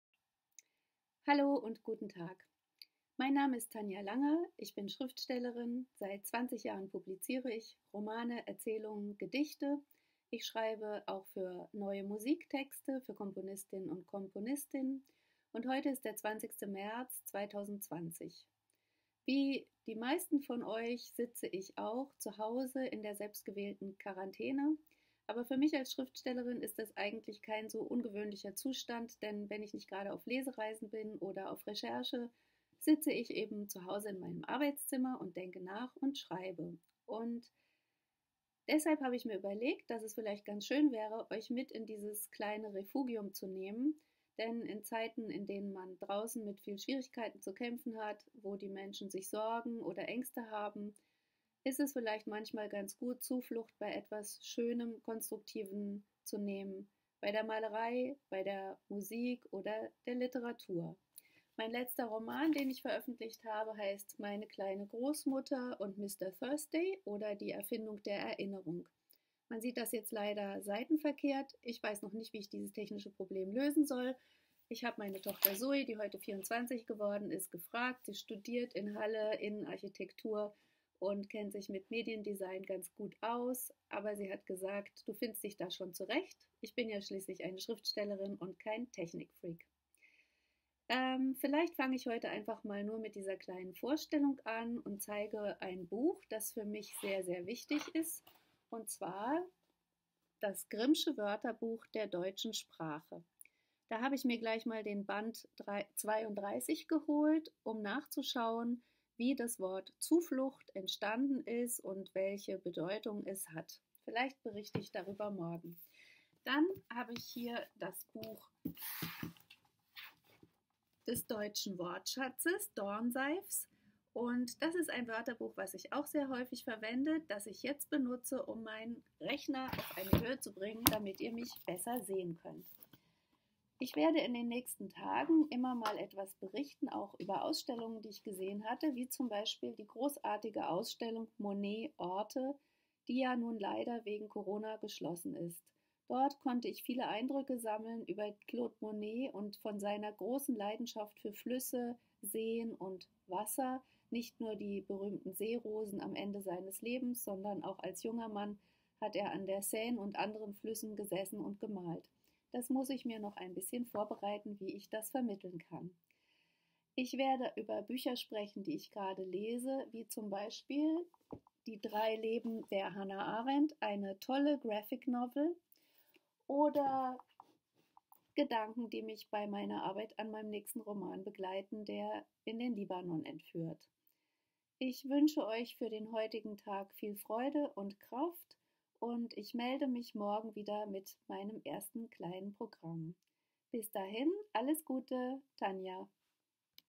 Tanja Langer: Zuflucht im Schönen
Tanja Langer: Zuflucht im Schönen - ein Corona-Programm von und mit der Schriftstellerin Tanja Langer, die aus dem Refugium ihrer Arbeit berichtet, über Malerei, Musik und Literatur, die sie begleiten. Zur Aufmunterung und zum Nachdenken in schwierigen Zeiten.